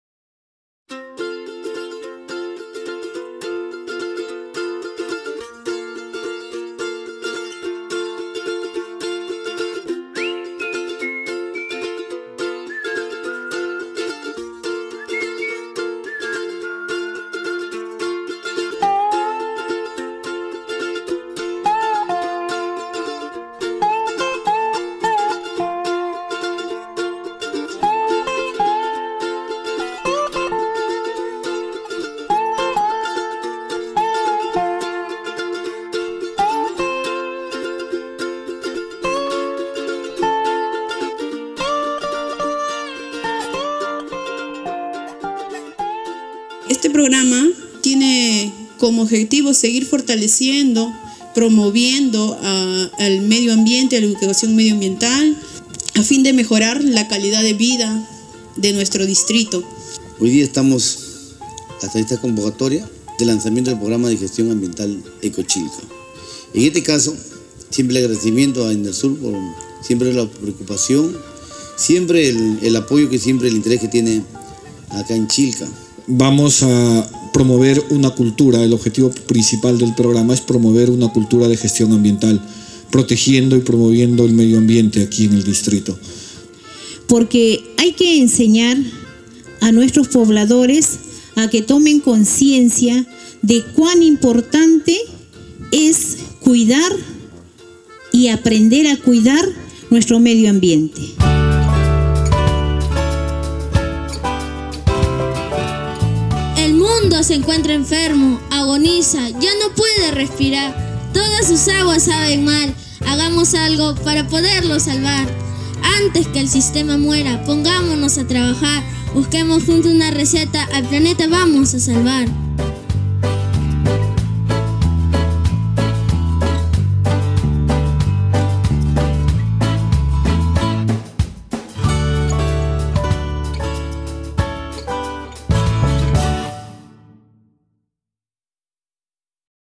Programa Ecochilca
Inauguración de la IV edición del programa de gestión ambiental de EnerSur denominado "EcoChilca, Juntos Protegemos el Medio Ambiente".